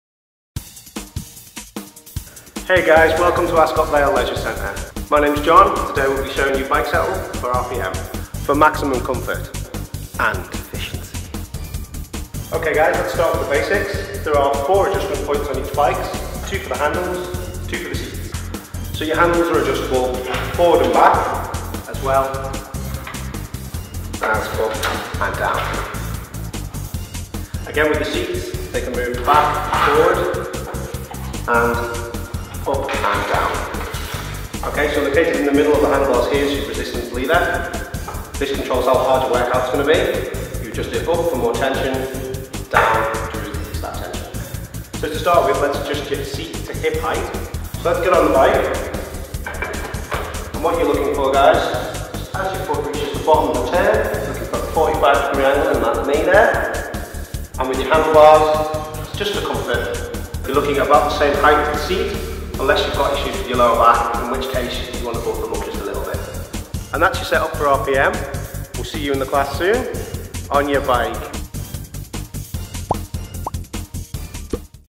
How to set up an RPM Bike | ASCOT VALE LEISURE CENTRE
Jonathan shows how to adjust and get ready for a cycling session at AVLC